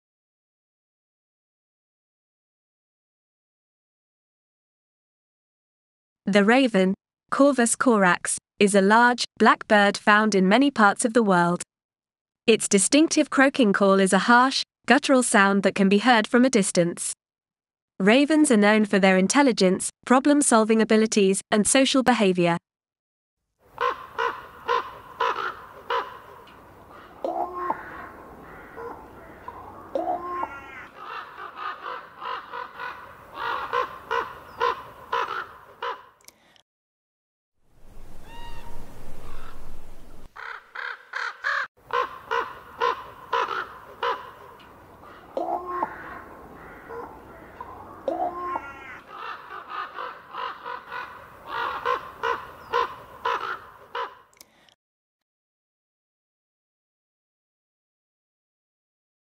EP23 The Raven - Bird Song
The Raven, with the scientific name Corvus corax, is a large bird that belongs to the Corvidae family. They are widely distributed across the Northern Hemisphere, from Europe and Asia to North America. Ravens are known for their intelligence, problem-solving abilities, and their distinctive vocalizations.

Ravens are large birds, typically measuring around 60-70cm in length with a wingspan of 120-150cm. They have a glossy black plumage with a purplish-blue iridescence in the sunlight. They have a distinctive wedge-shaped tail and a large, strong bill. Ravens are known for their deep, croaking call, which can carry for long distances.

Ravens are highly adaptable birds and can be found in a wide range of habitats, from mountains and forests to deserts and urban areas. They are opportunistic feeders and will eat a wide variety of food, including carrion, insects, fruit, and small mammals.

Ravens are known for their intelligence and problem-solving abilities. They have been observed using tools, such as sticks, to access food sources, and have also been observed working together to achieve a goal. They are also known to be curious and have been observed investigating new objects and environments.

In terms of reproduction, ravens typically form long-term monogamous pairs. They build large nests of sticks and twigs in trees, on cliffs, or on other high structures. The female typically lays 3-7 eggs, and both parents are involved in caring for the young. The young are cared for in the nest for around 6-7 weeks before fledging.

In summary, the Raven, with the scientific name Corvus corax, is a large and intelligent bird known for its deep, croaking call, glossy black plumage, and problem-solving abilities. They are opportunistic feeders and can be found in a wide range of habitats. Ravens typically form long-term monogamous pairs and build large nests of sticks and twigs to raise their young.